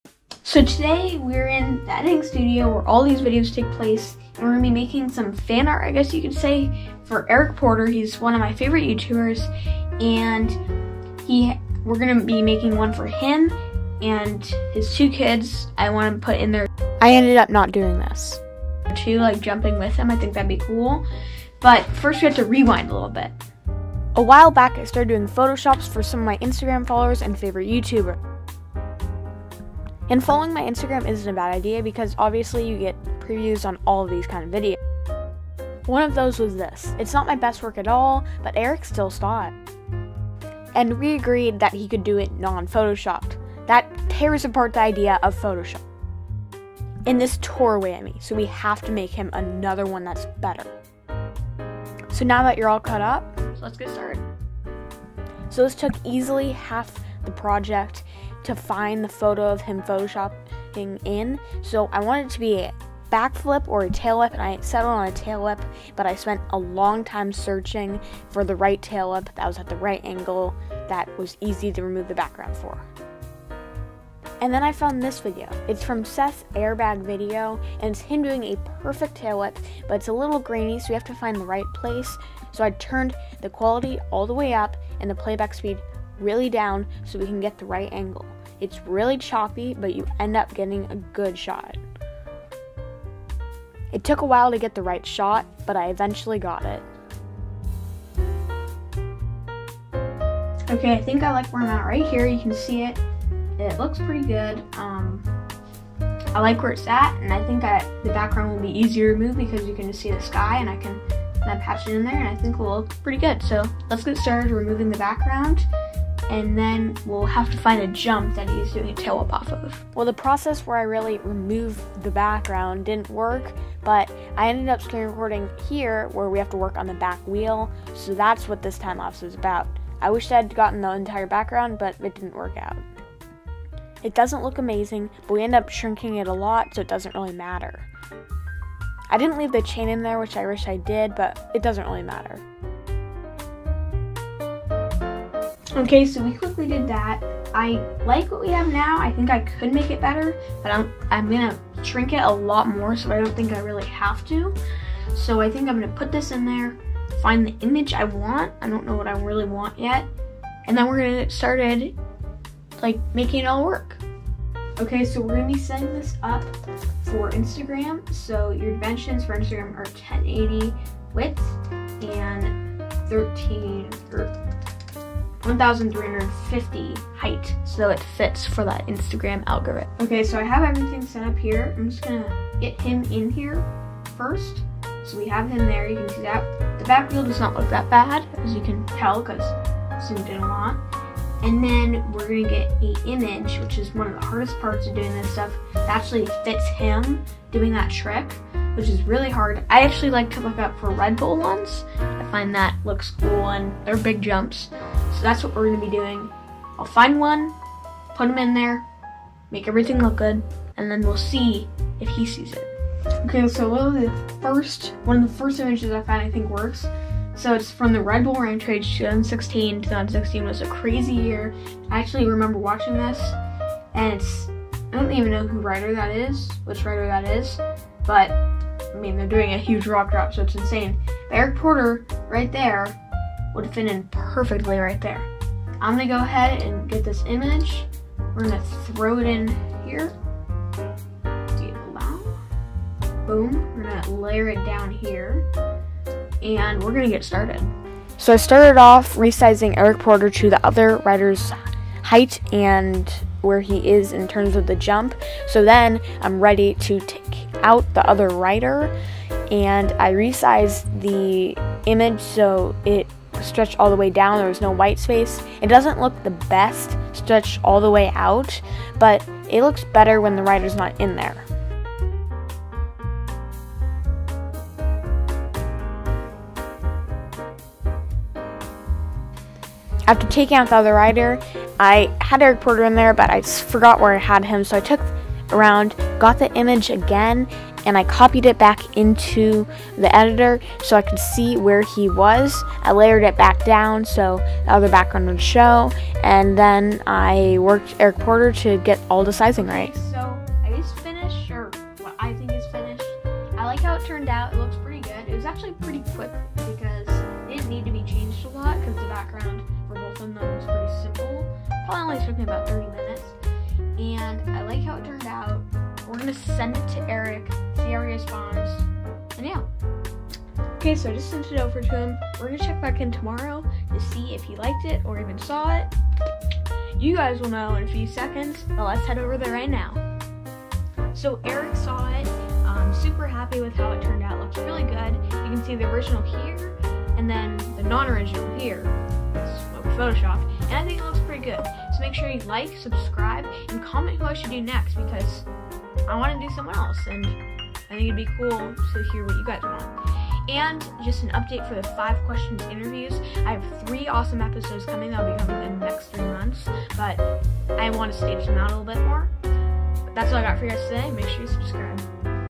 I Photoshopped Eric Porter Into The RedBull Rampage And He Saw It!
Today we make some photoshopped art for @Eric Porter!